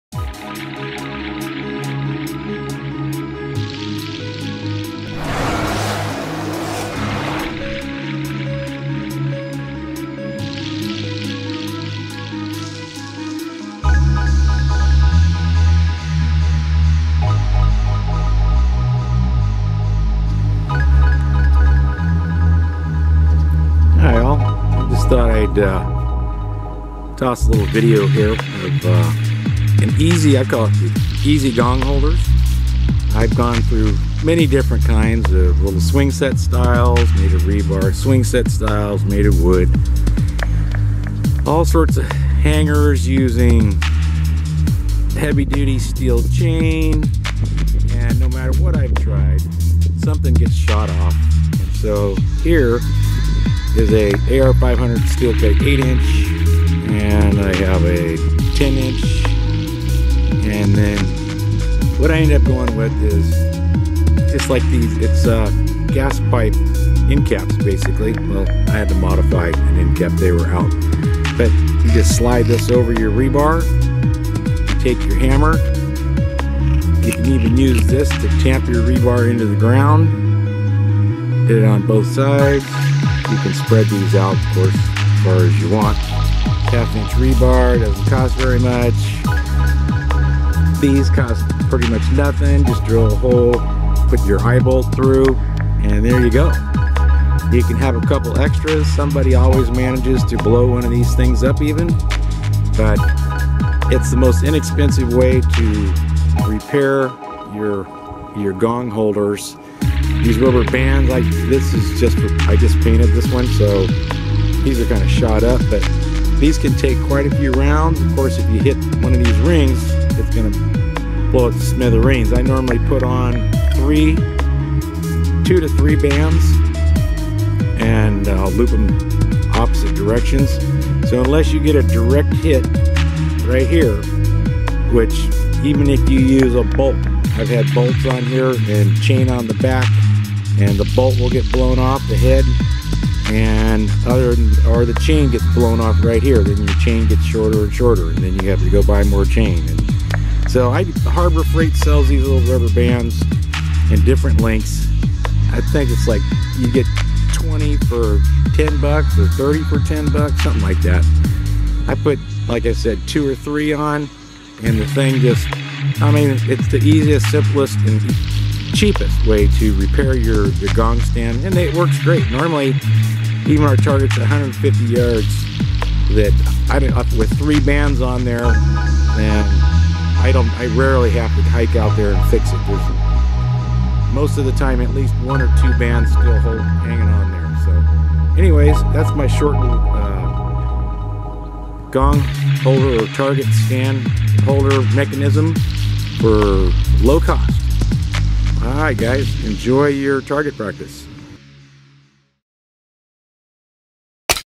Steel Target stands DIY
DIY steel or other target stand and hanger. I have tried most everything on YouTube as ideas for a DIY steel target gong holder. The weak link pun intended is the connection. Every chain, and all connections I have tried get blown away, some instantly some in a few sessions. Sure they say don't miss. LOL they all miss depending on what counts as a hit. So this is what works well for me over the past 6 years. Give it a try... it will save you time and money... $$ Fast cheap and spend more time hitting the target than repairing it. Everyone laughs that rubber lasts longer... try it then laugh. :P